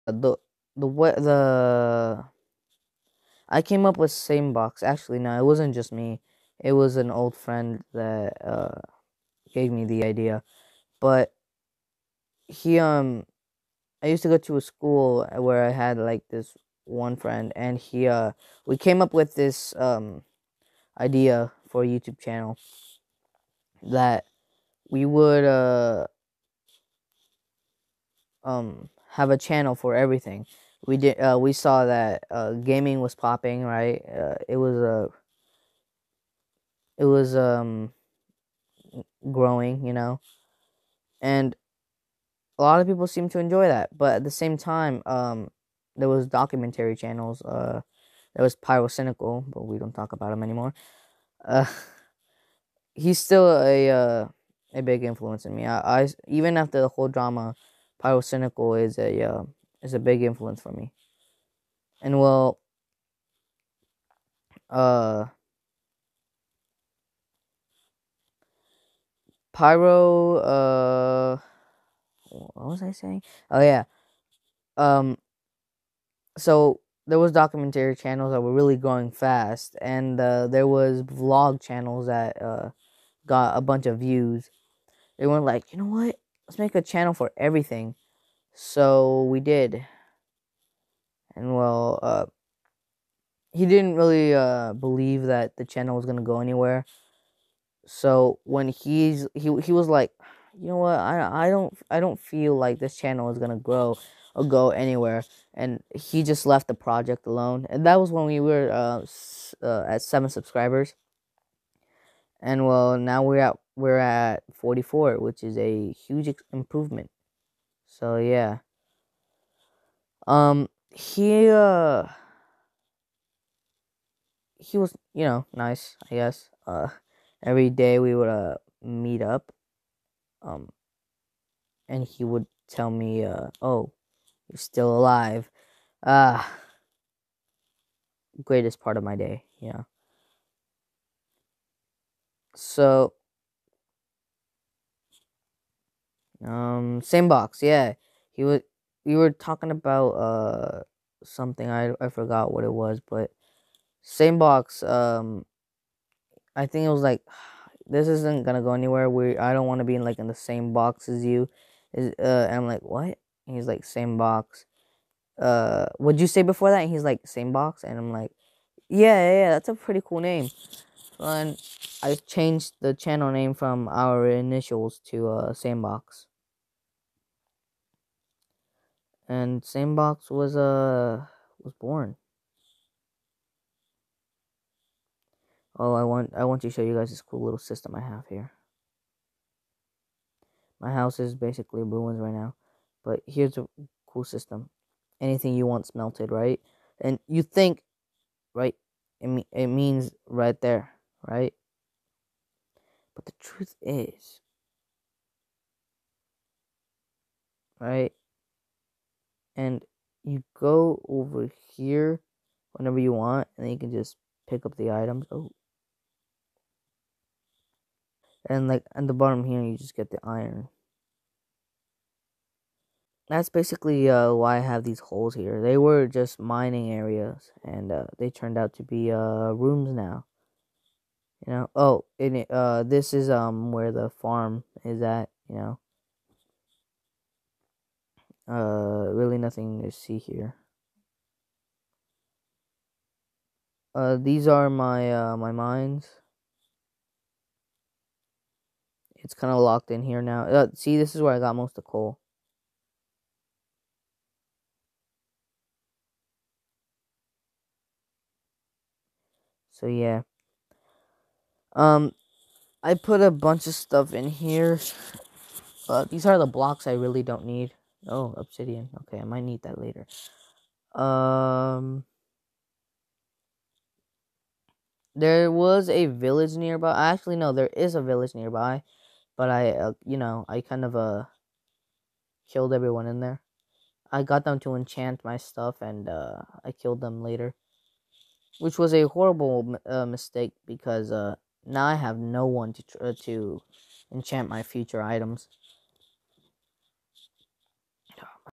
Explaining how same box came to be
Explaining my channel name